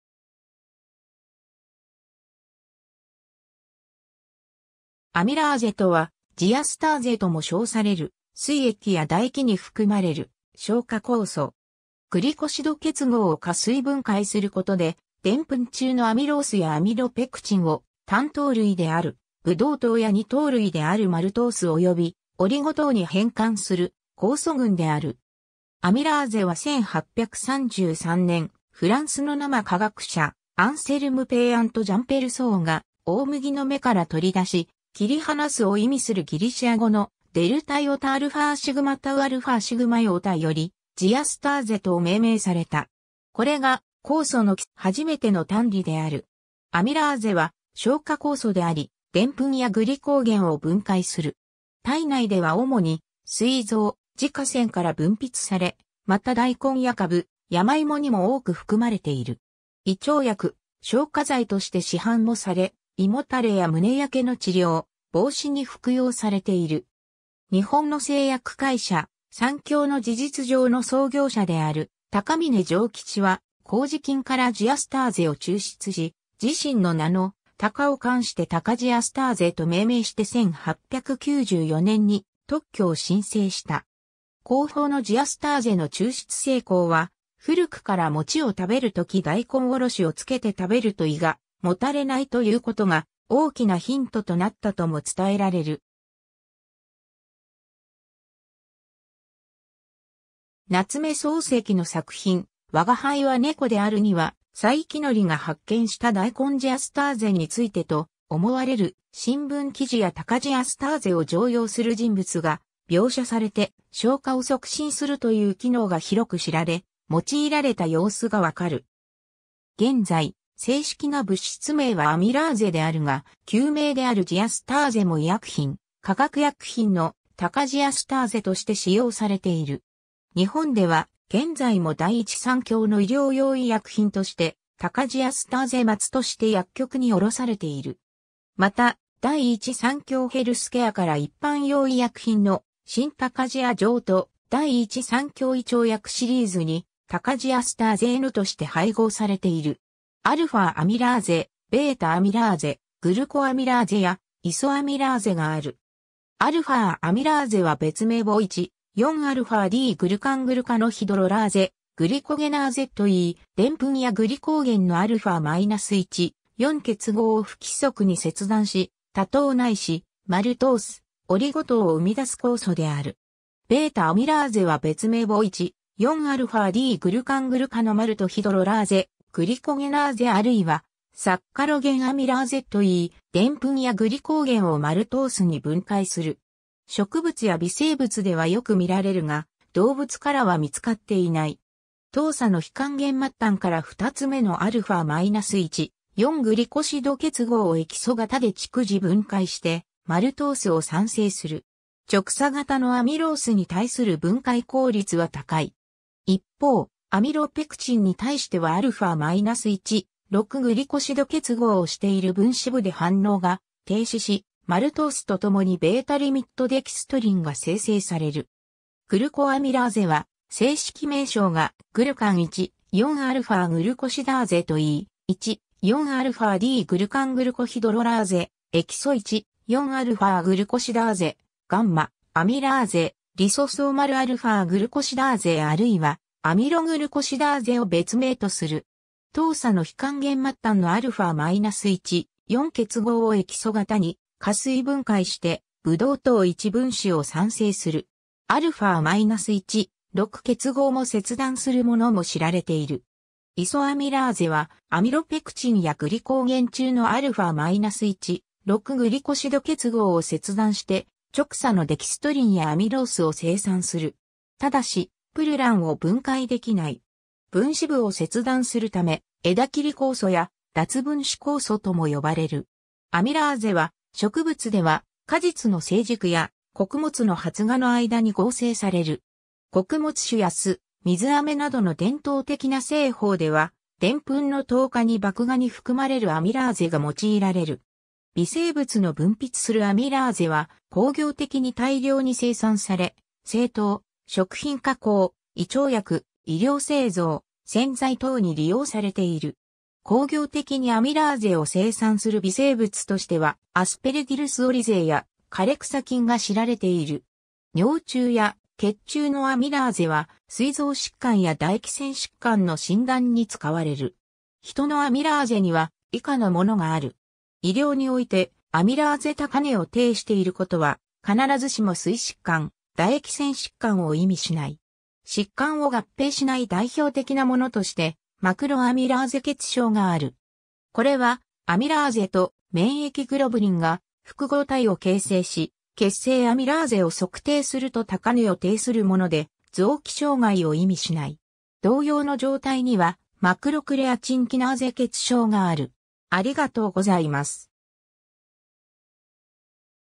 アミラーゼ
アミラーゼ (amylase)とはジ(ヂ)アスターゼとも称される、膵液や唾液に含まれる消化酵素。
グリコシド結合を加水分解することでデンプン(ラテン語"amylum")中のアミロースやアミロペクチンを、単糖類であるブドウ糖や二糖類であるマルトースおよびオリゴ糖に変換する酵素群である。
 アミラーゼは1833年、フランスの生化学者、アンセルム・ペイアン (Anselme Payen) とジャン・ペルソー (Jean F. Persoz) が大麦の芽から取り出し、「切り離す」を意味するギリシア語の “διαστασις” より「ジ(ヂ)アスターゼ」と命名された。
これが酵素の初めての単離である。
 アミラーゼは消化酵素であり、デンプンやグリコーゲンを分解する。
体内では主に、膵臓、耳下腺（唾液腺）から分泌され、またダイコンやカブ、ヤマイモにも多く含まれている。
胃腸薬、消化剤として市販もされ、胃もたれや胸焼けの治療、防止に服用されている。
 日本の製薬会社三共の事実上の創業者である高峰譲吉は、麹菌からジアスターゼを抽出し、自身の名の「タカ」を冠してタカジアスターゼと命名して1894年（明治27年）に特許を申請した。
高峰のジアスターゼ（アミラーゼ）の抽出成功は古くから餅を食べるとき大根おろしをつけて食べると胃がもたれないと言う事が大きなヒントとなったとも伝えられる。
 夏目漱石の作品『吾輩は猫である』には、佐伯矩が発見した大根ジアスターゼについてと思われる新聞記事やタカジアスターゼを常用する人物が描写されて、消化を促進するという機能が広く知られ用いられた様子がわかる。
 現在、正式な物質名はアミラーゼであるが、旧名であるジアスターゼも医薬品/化学薬品の『タカジアスターゼ』として使用されている。
 日本では現在も第一三共の医療用医薬品として「タカヂアスターゼ末」として薬局に卸されている（主に解熱鎮痛剤や整腸剤など他の散剤と混合して使うが、処方箋医薬品ではないため零売が可能）。
また、第一三共ヘルスケアから一般用医薬品（胃腸薬）の「新タカヂア錠」と「第一三共胃腸薬」シリーズにタカヂアスターゼNとして配合されている。
 α-アミラーゼ、β-アミラーゼ、グルコアミラーゼやイソアミラーゼがある。
 α-アミラーゼは別名を1,4-α-D-グルカングルカノヒドロラーゼ、グリコゲナーゼといい、デンプンやグリコーゲンのα-1,4-結合を不規則に切断し、多糖ないしマルトース、オリゴ糖を生み出す酵素である。
 β-アミラーゼは別名を1,4-α-D-グルカングルカノマルトヒドロラーゼ、グリコゲナーゼあるいはサッカロゲンアミラーゼといい、デンプンやグリコーゲンをマルトース(麦芽糖)に分解する。
植物や微生物ではよく見られるが、動物からは見つかっていない。
糖鎖の非還元末端から二つ目のα-1,4-グリコシド結合をエキソ型で逐次分解してマルトースを産生する。
直鎖型のアミロースに対する分解効率は高い。
一方、アミロペクチンに対してはα-1,6-グリコシド結合をしている分枝部で反応が停止し、マルトースとともにβリミットデキストリンが生成される。
 グルコアミラーゼは正式名称がグルカン1,4-α-グルコシダーゼといい、1,4-α-D-グルカングルコヒドロラーゼ、エキソ1,4-α-グルコシダーゼ、γ-アミラーゼ、リソソーマルα-グルコシダーゼあるいはアミログルコシダーゼを別名とする。
糖鎖の非還元末端のα-1,4-結合をエキソ型に加水分解してブドウ糖1分子を産生する。
α-1,6-結合も切断するものも知られている。
 イソアミラーゼはアミロペクチンやグリコーゲン中のα-1,6-グリコシド結合を切断して直鎖のデキストリンやアミロースを生産する。
ただし、プルランを分解できない。
分枝部を切断するため、枝切り酵素や脱分枝酵素とも呼ばれる。
 アミラーゼは、植物では果実の成熟や穀物の発芽の間に合成される。
穀物酒や酢、水あめなどの伝統的な製法ではデンプンの糖化に麦芽に含まれるアミラーゼが用いられる。
 微生物の分泌するアミラーゼは工業的に大量に生産され、製糖、食品加工、胃腸薬、衣料製造、洗剤等に利用されている。
工業的にアミラーゼを生産する微生物としてはアスペルギルス・オリゼーや枯草菌が知られている。
 尿中や血中のアミラーゼは、膵臓疾患や唾液腺疾患の診断に使われる。
 ヒトのアミラーゼには以下のものがある。
 医療においてア...